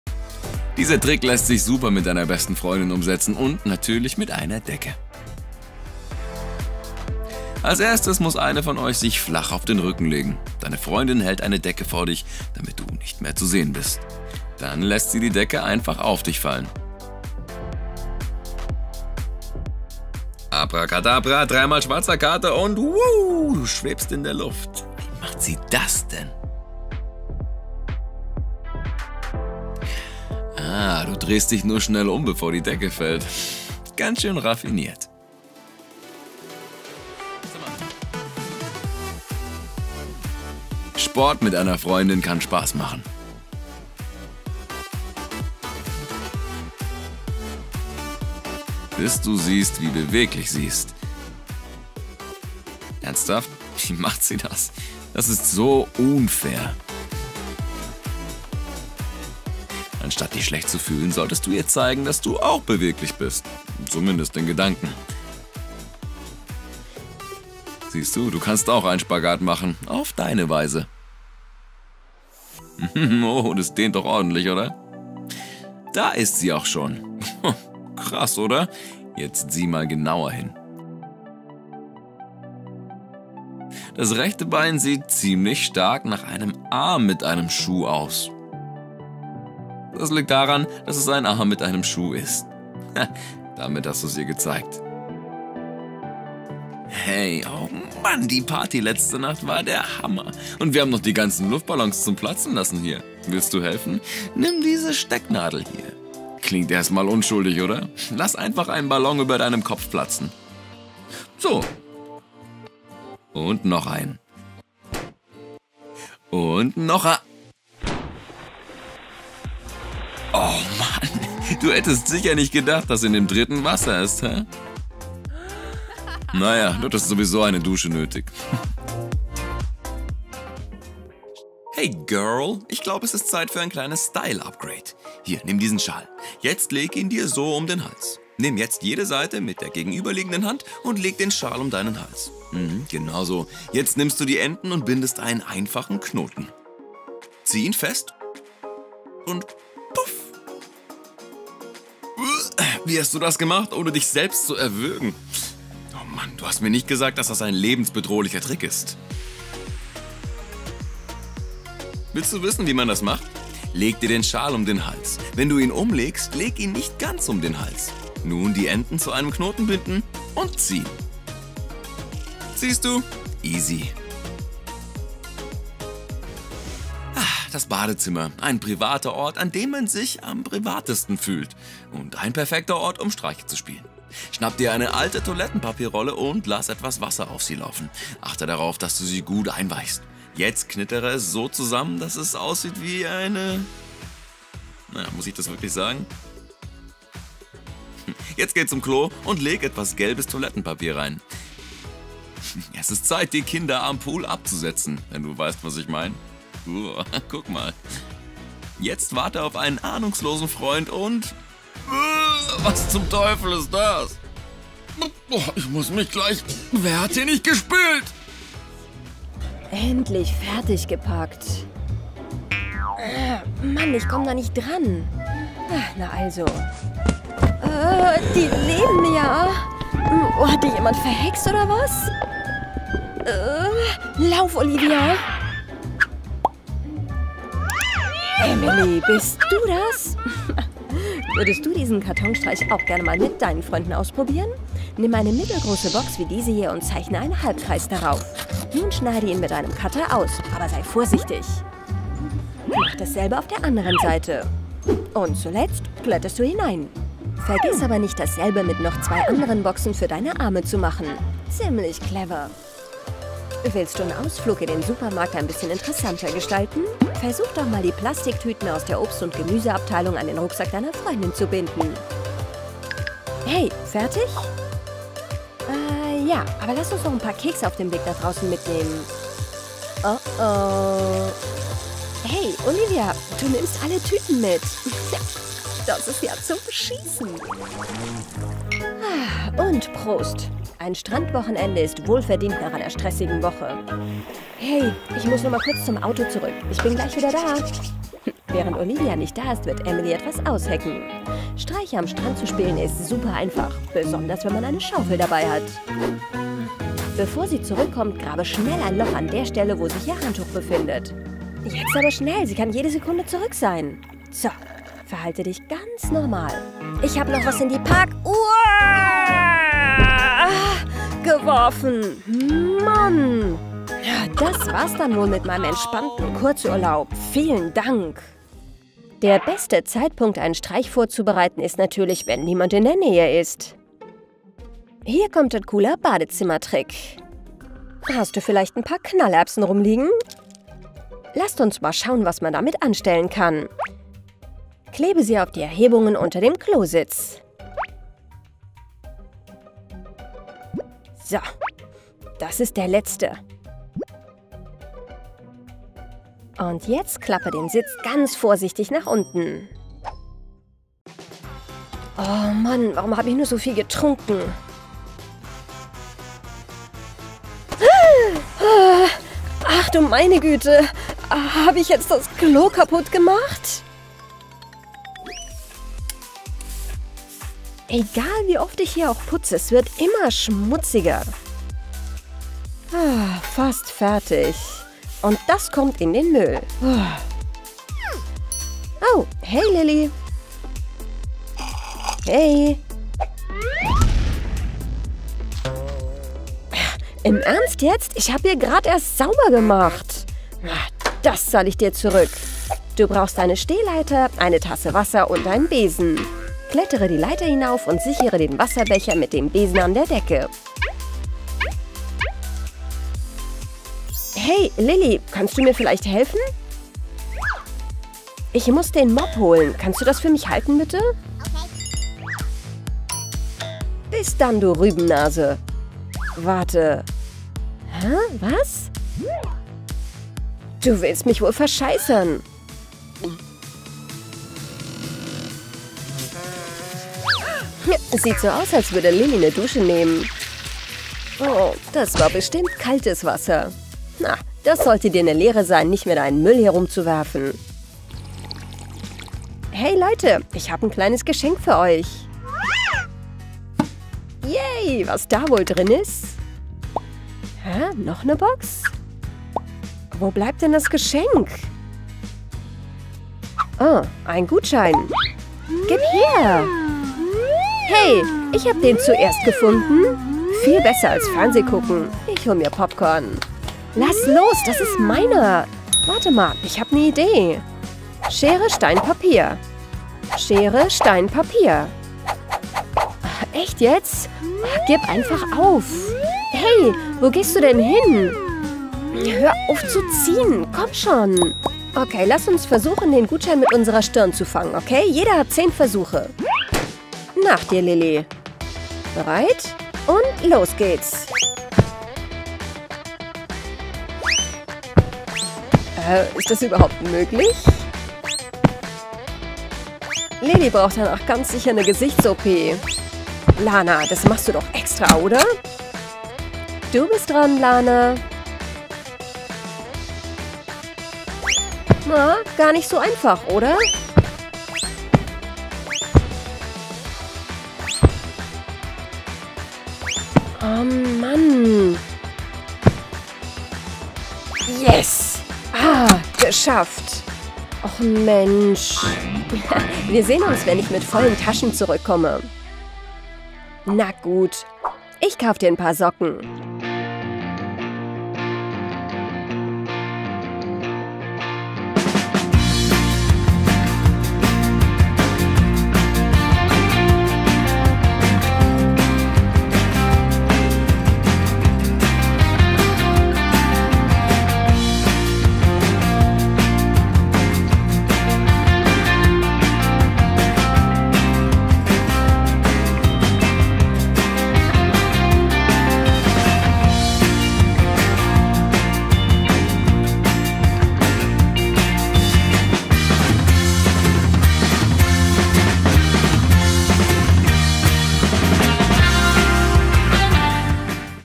DIE BESTEN CHALLENGES UND STREICHE FÜR FREUNDE || Wasserballon Challenge by 123 GO! GOLD
Weiß sie, dass in diesem Ballon Wasser ist? Sei dabei beim hochexplosiven Wasserballon-Streich! 😱 Diese Challenge ist einfach fantastisch!

Genieße unsere brandneuen, lustigen Streiche und spannenden Herausforderungen in diesem verrückten Comedy-Video!

Du wirst lernen, wie man schwebt und beeindruckende Zaubertricks vorführt, wie man einen Schal bindet, um umwerfend auszusehen und du siehst den lustigsten Streich von Vicky!

Hab Spaß mit diesen Streichen und schocke deine Freunde ein wenig. Stellt euch den neuen Herausforderungen!

GOLD und klicke auf die Glocke 🔔, damit du keines unserer Videos verpasst.

Bis bald!

ZEITSTEMPEL:
00:10 Zaubertrick - Levitation
01:08 Magie endlich enthüllt!
01:37 Die Wasserballon-Challenge
03:24 Der Toilettenpapier-Streich
05:00 Streiche für Freunde - gemeinsam Spaß haben